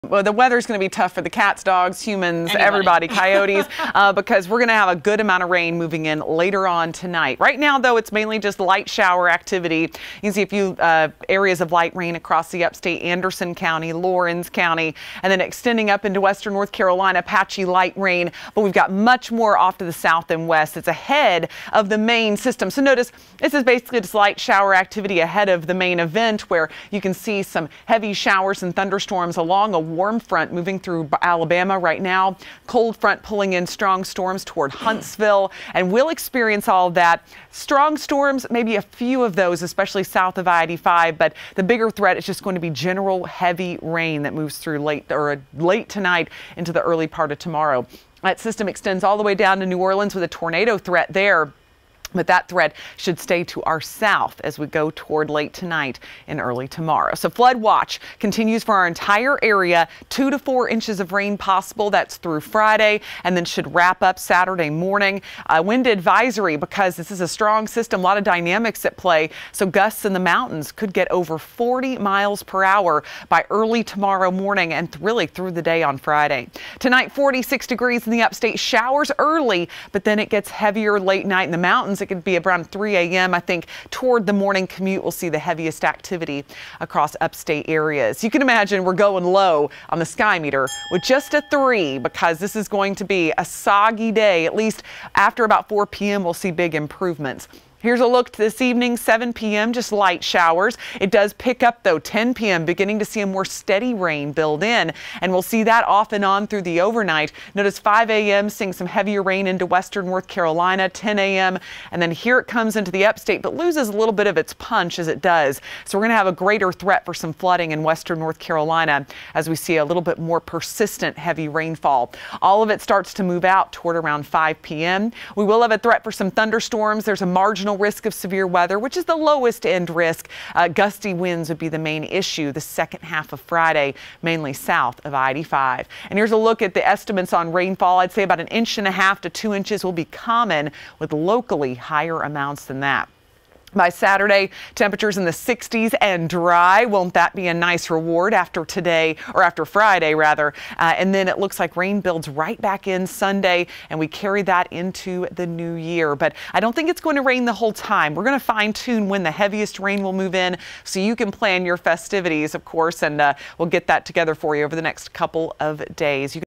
Kendra: Rain pushing in Thurs. night into Friday
Kendra Kent's Dec. 27, 2018 weather forecast.